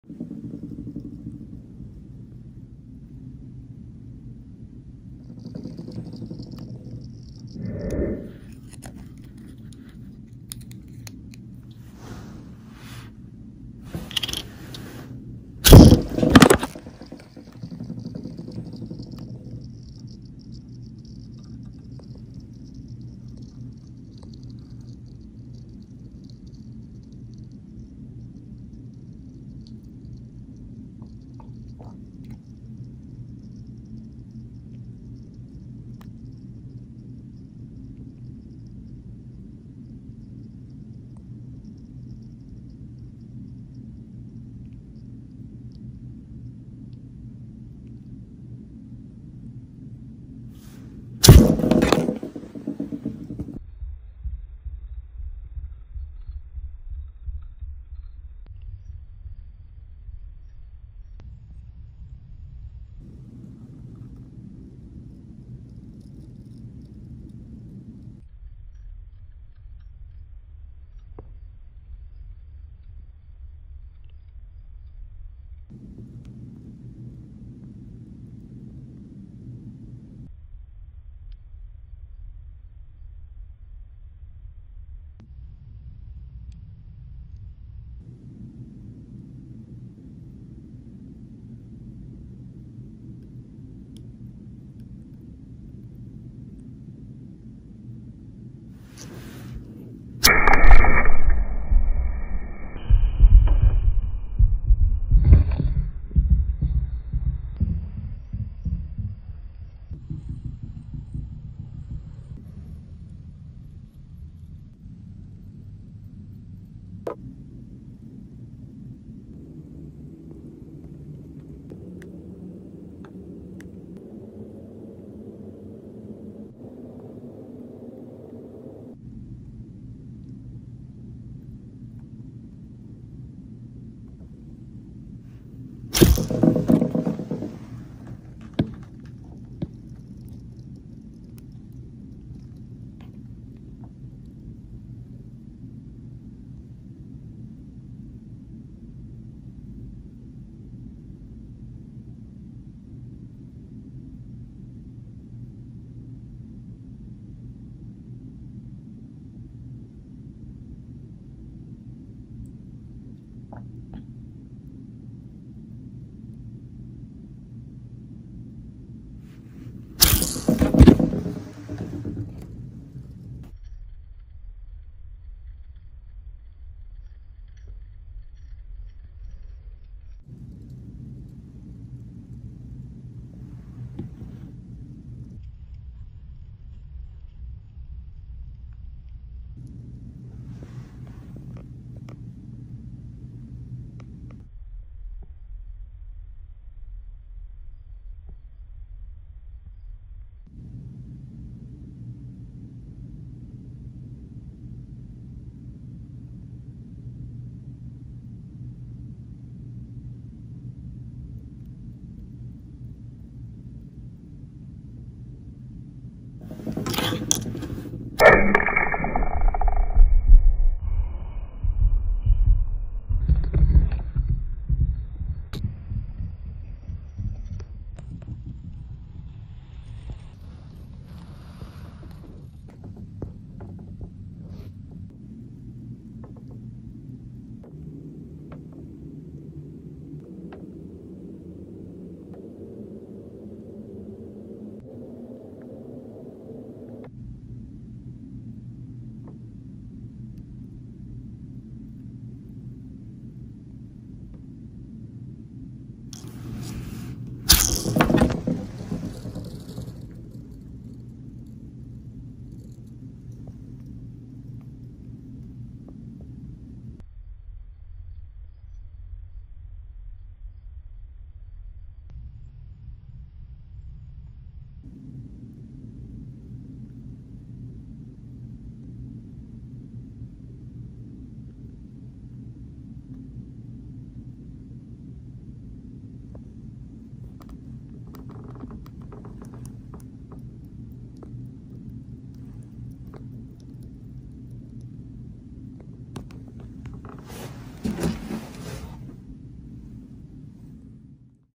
ASMR - Spinning Taylor Swift Photo on Bottle Cap
ASMR - Spinning Taylor Swift Picture on a bottle cap plays at 500% sound and random intervals in slow motion.